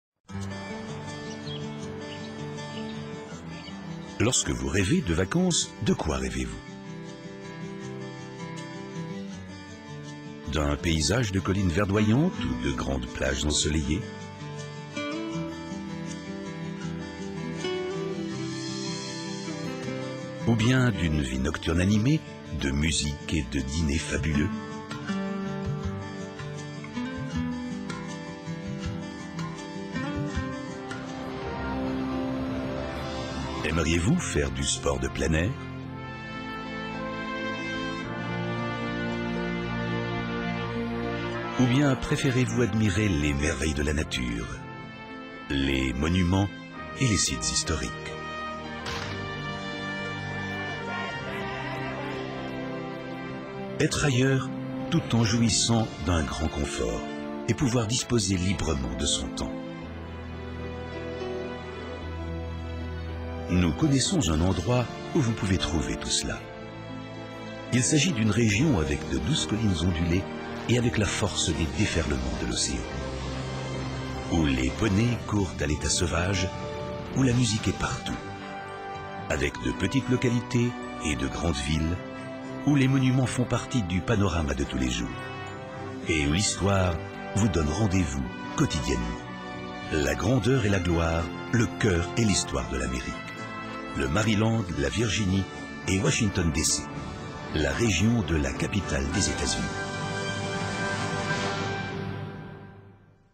Washington DC and the Capital Region USA - French Short
Explore the National Capital region of the United States with this short promotional video.

ASAA Multimedia
Producer: Andrew Aurbach